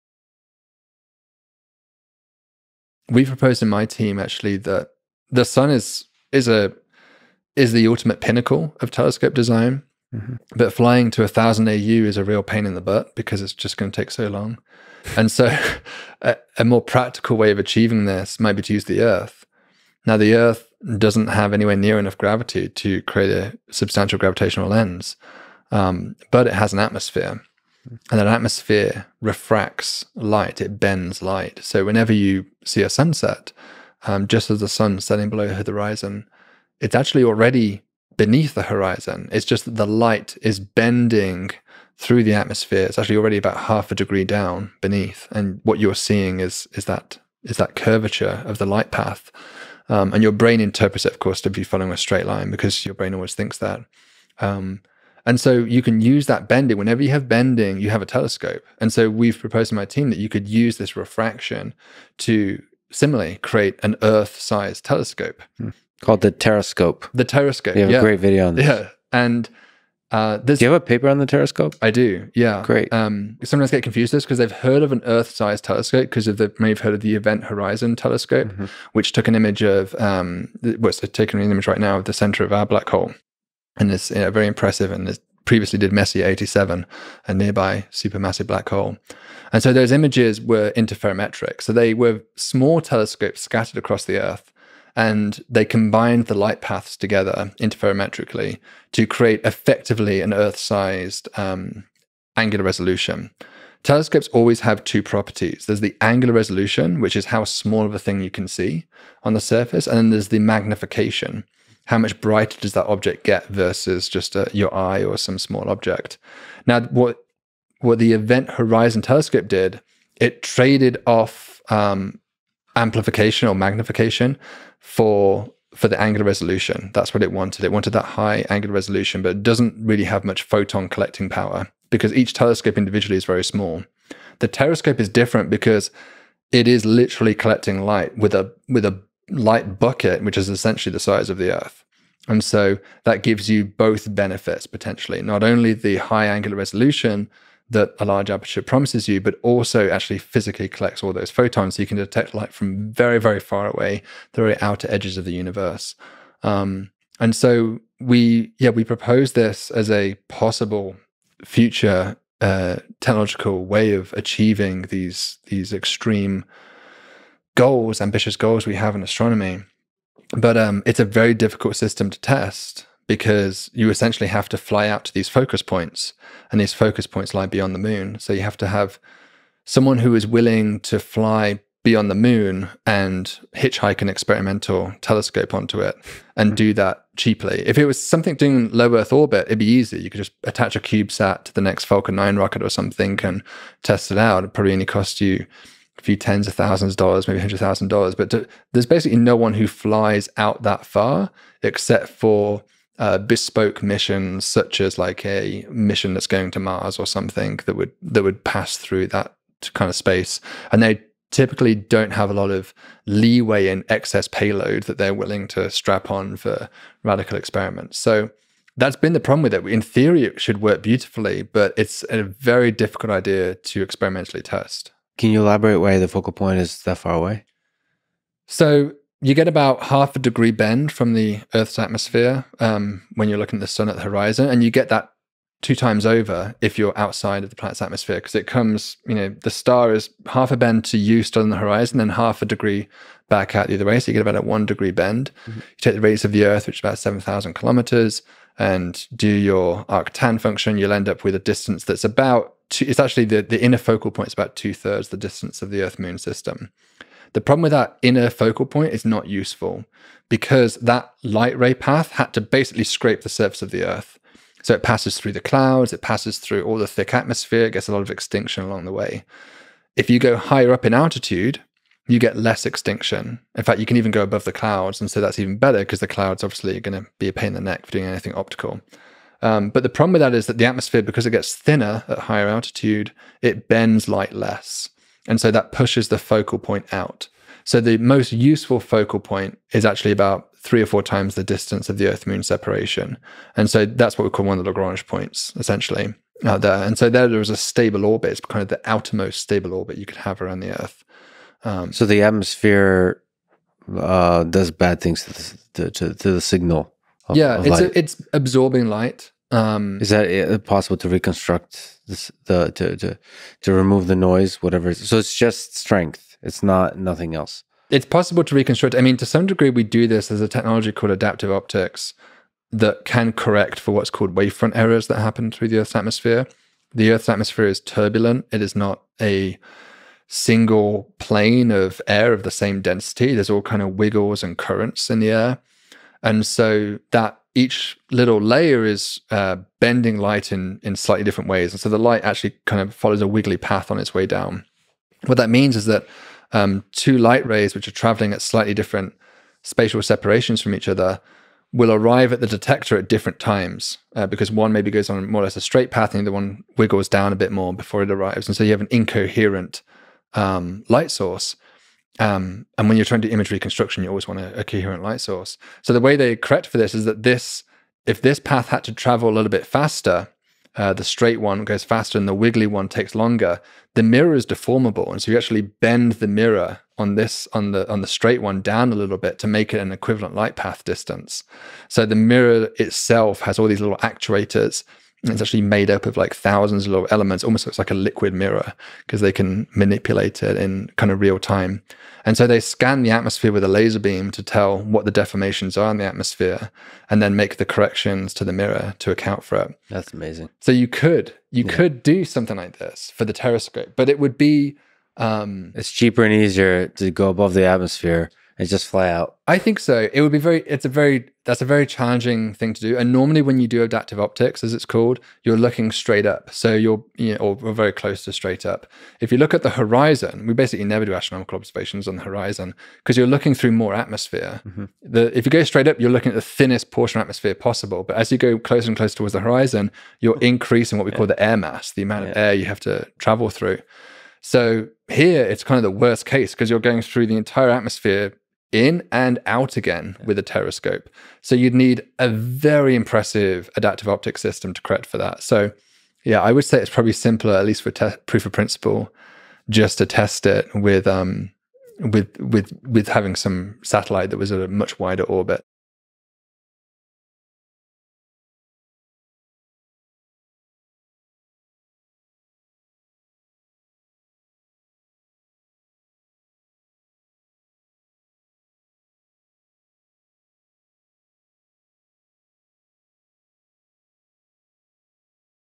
Astronomer explains the Terrascope: Earth-sized telescope | David Kipping and Lex Fridman
GUEST BIO:
David Kipping is an astronomer at Columbia University, director of the Cool Worlds Lab, and host of the Cool Worlds YouTube channel.